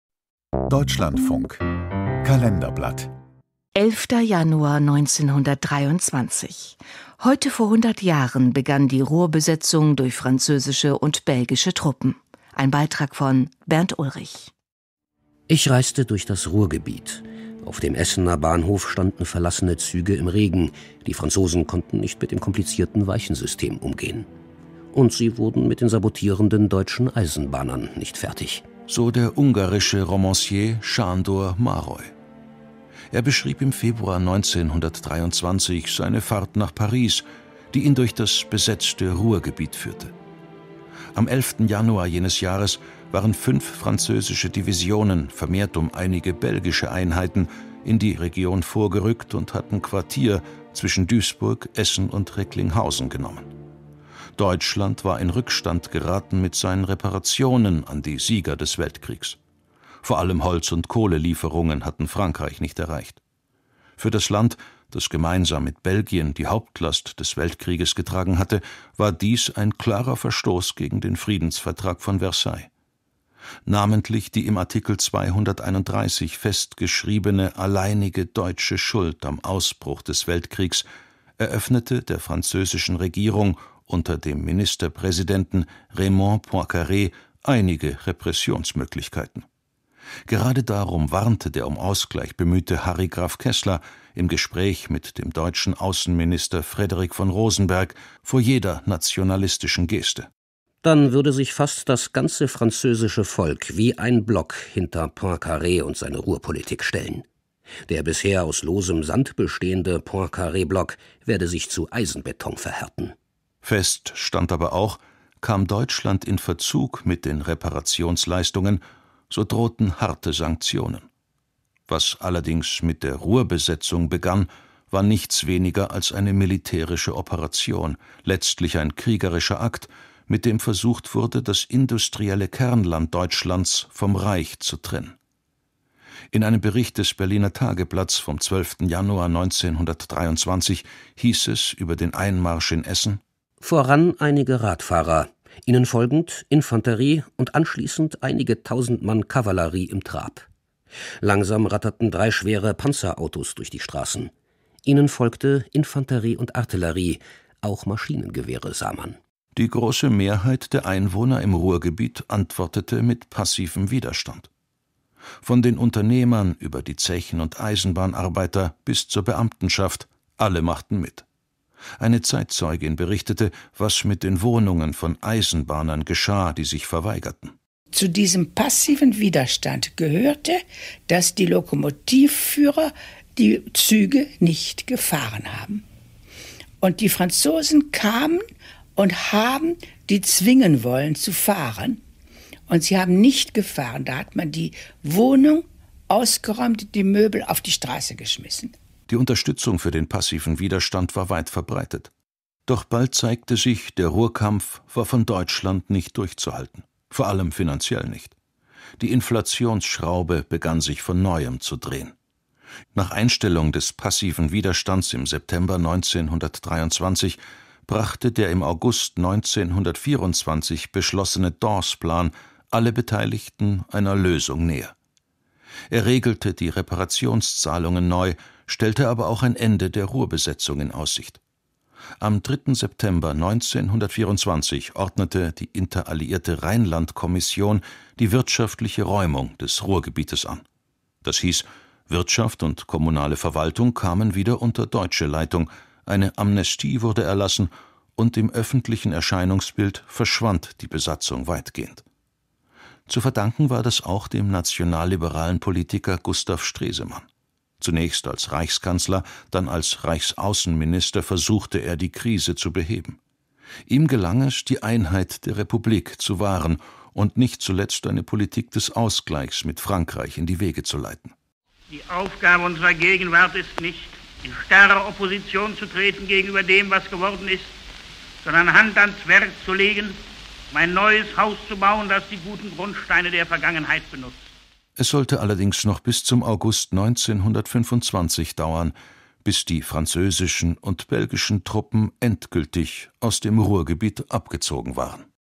DLF 11.01.1923 Ruhrbesetzung begann. Kriegerischer Akt gegen Deutschlands industriellen Kern
Ab dem 11. Januar 1923 marschierten französische und belgische Truppen ins Ruhrgebiet, besetzten das Land zwischen Duisburg und Dortmund, um es wirtschaftlich auszubeuten und das im Ersten Weltkrieg unterlegene Deutsche Reich zu schwächen.

Von Bernd Ulrich